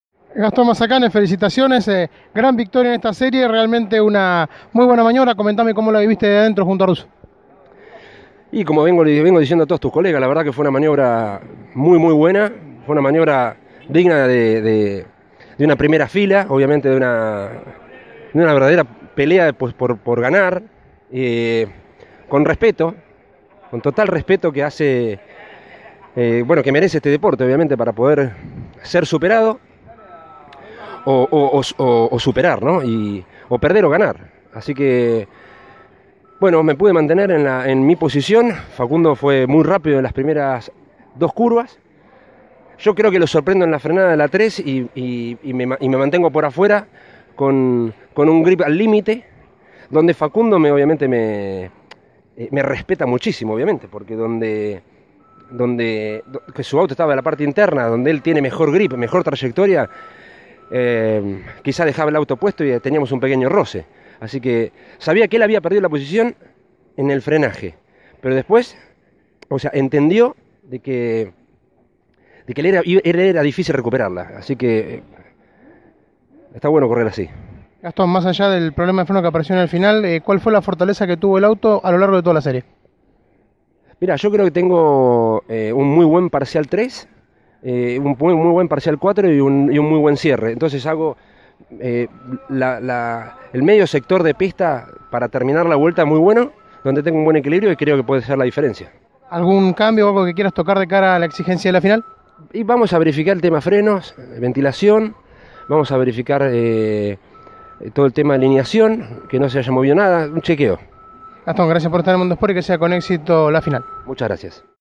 Gastón Mazzacane - Serie 2 TC CDU 2019
Luego de quedarse con la segunda serie del TC en el autódromo de Concepción del Uruguay, Gastón Mazzacane pasó por los micrófonos de Mundo Sport y comentó sus sensaciones.